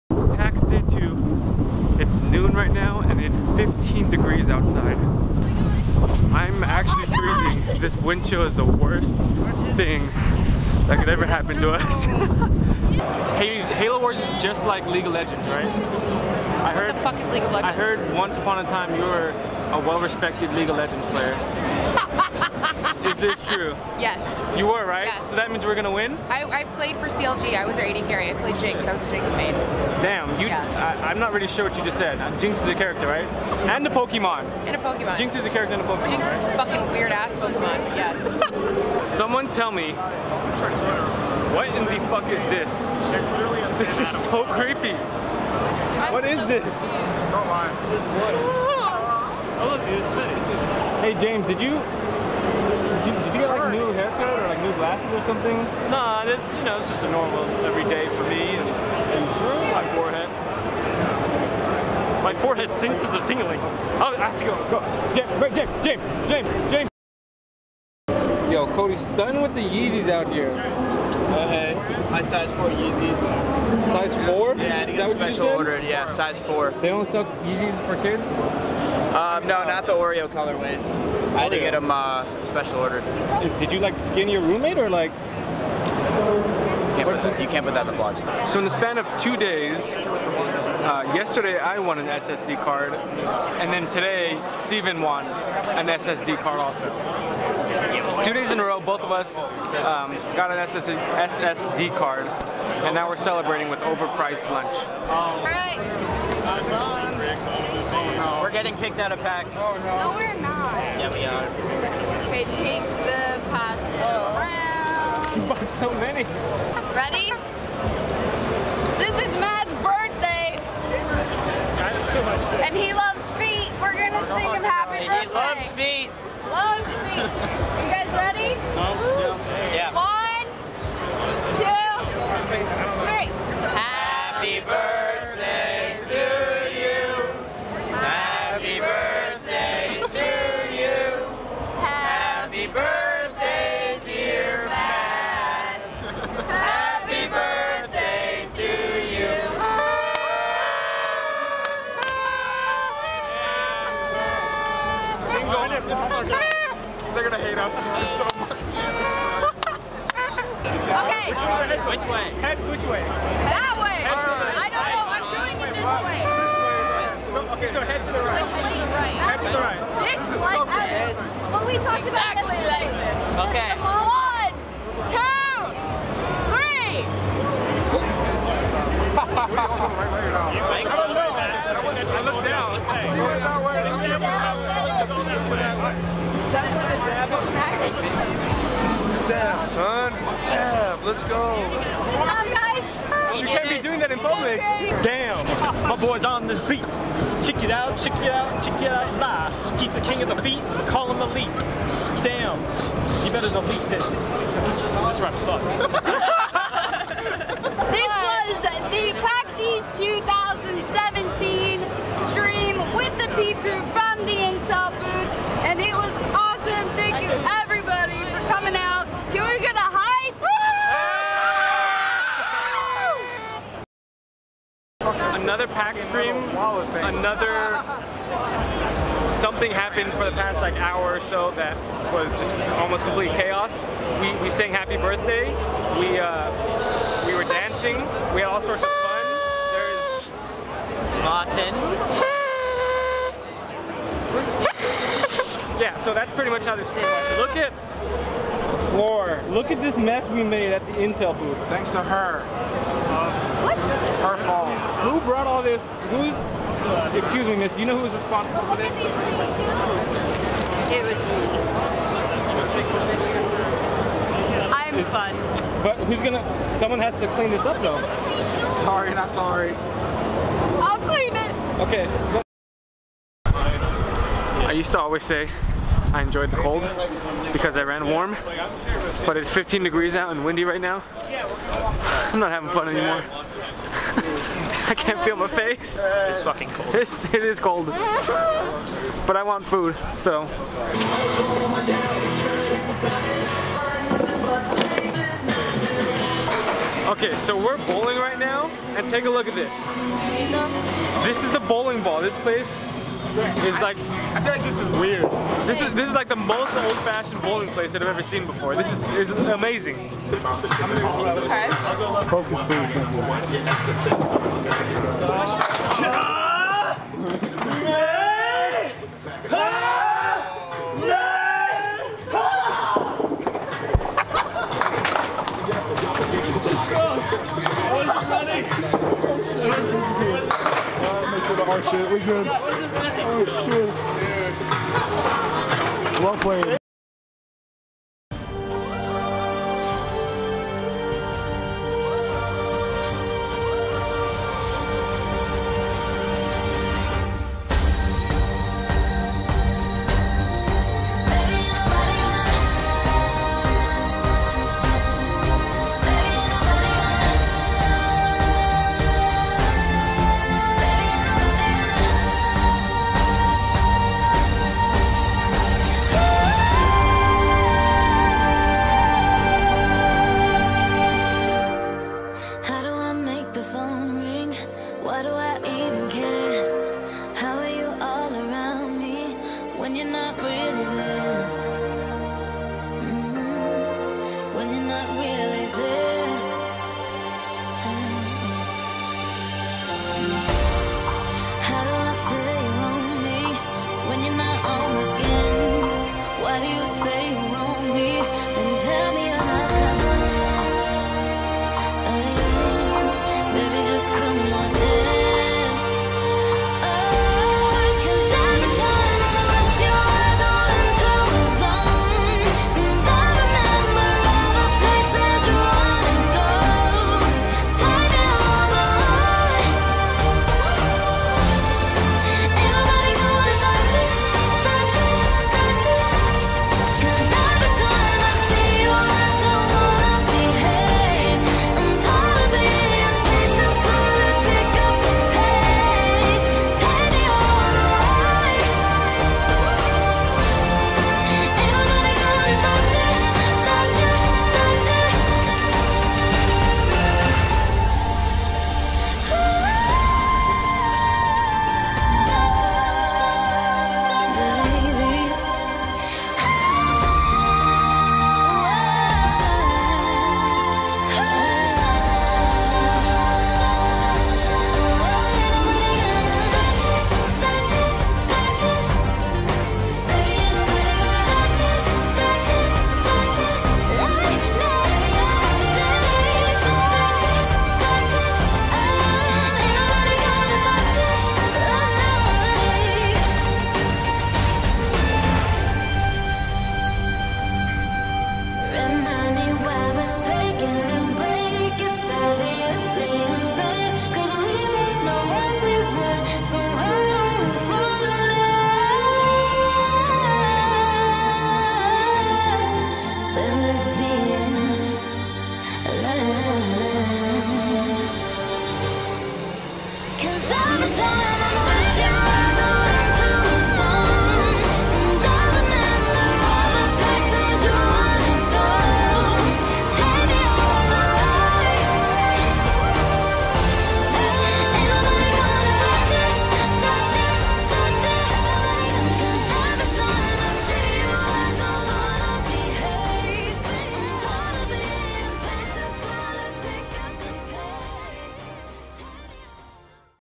Complete Chaos- PAX East Day 2
This might not be your "conventional" PAX East video- but then again what is? Hung out with friends today, played some video games, partied on stream. Just a normal peep crew pax if ya ask me. Also in typical fashion we somehow managed to mix dancing into bowling lmao

I'll be uploading daily vlogs @ 8am PST for the month of March!
Traveling from Los Angeles, CA - Portland, OR - Boston, MA- Manhattan, NY - The Philippines
Super excited for this upcoming month of travel :]